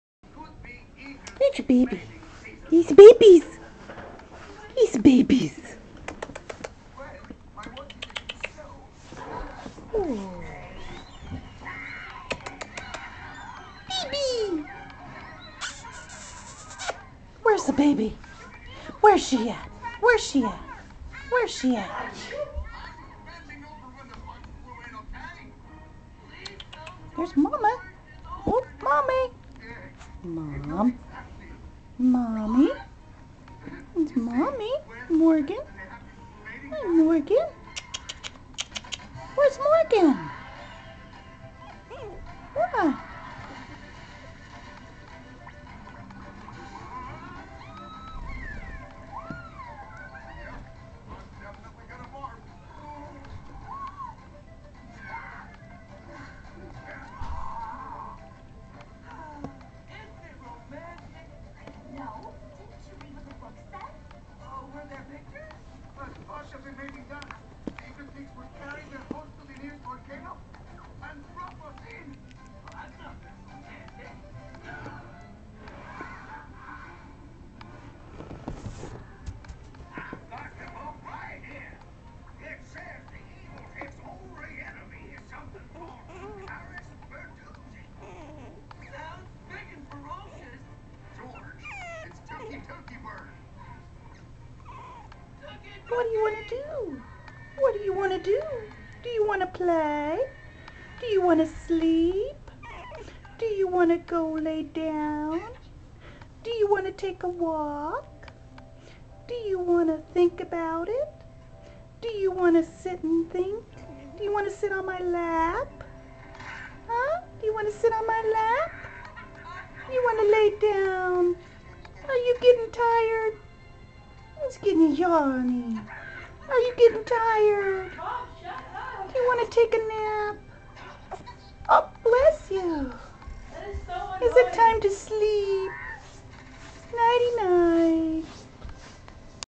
Papillon puppies line up and heads tilt side to side
In the middle of this video our papillon puppies line up next to each other and tilt their little heads as I ask them questions.